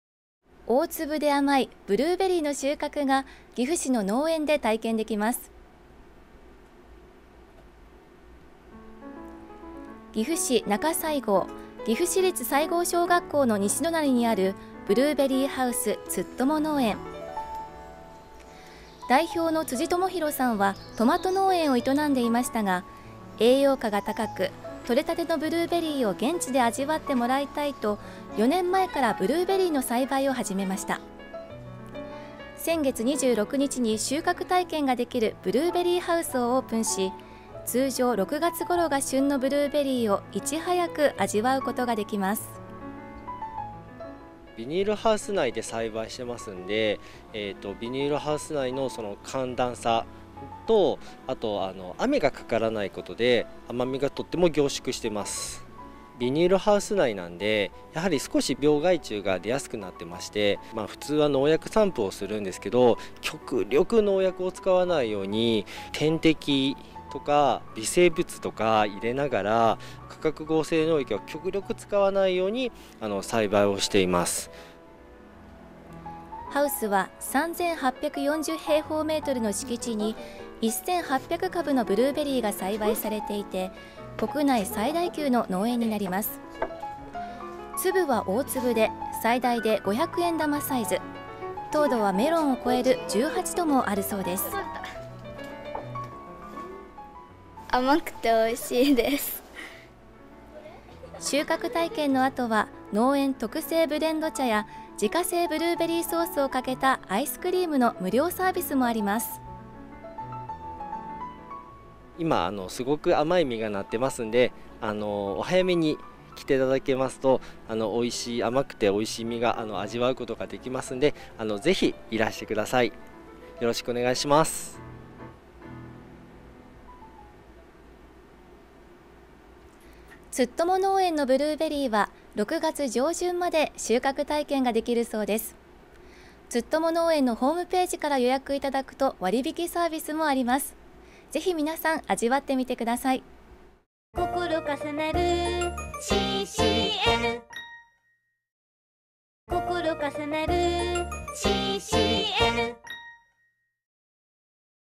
【岐阜のニュース】岐阜市に収穫体験ができるブルーベリーハウスがオープン（2022年4月1日放送）
岐阜県のケーブルテレビＣＣＮ（シーシーエヌ）の番組やＣＭ・ニュースを公開！

2022年3月に、岐阜市で収穫体験ができるブルーベリーハウスがオープンしました。
通常６月頃が旬のブルーベリーを一足早く味わうことができます。